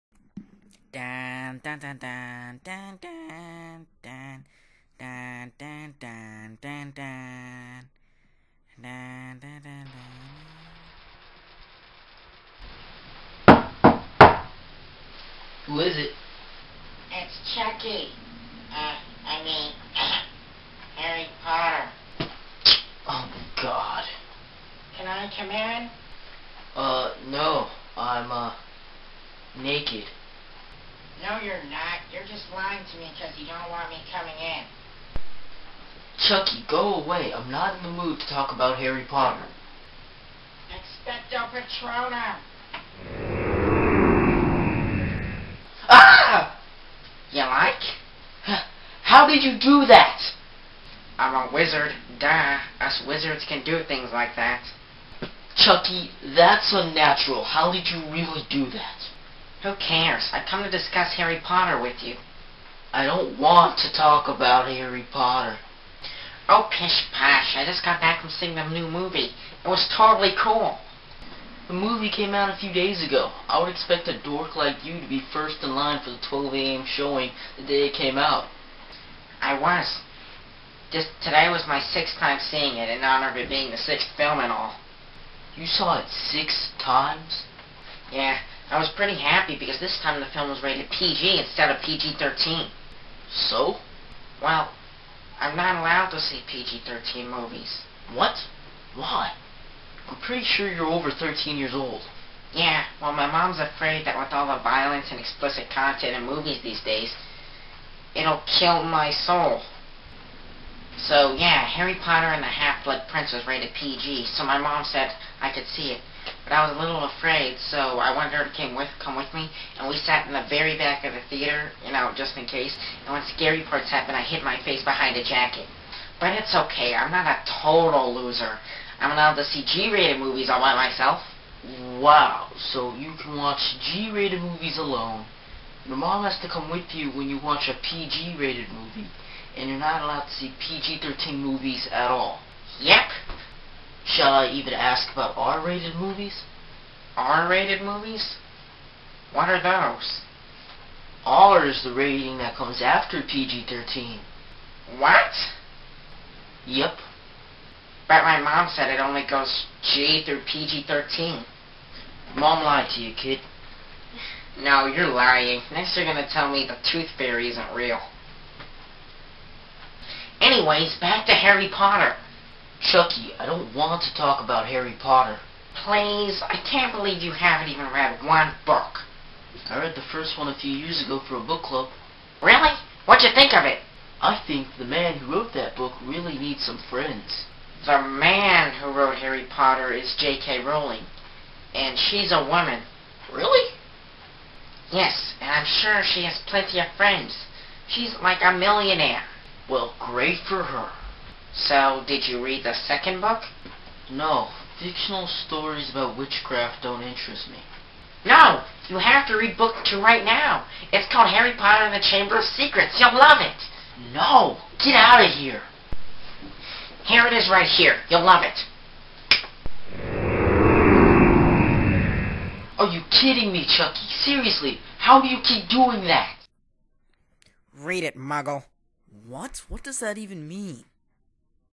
A Harry Potter Conversation
A comedic "review" of the new film, 'Harry Potter and the Half Blood Prince.'

Featuring another humorous conversation with Jon and Chuckie.

THX :)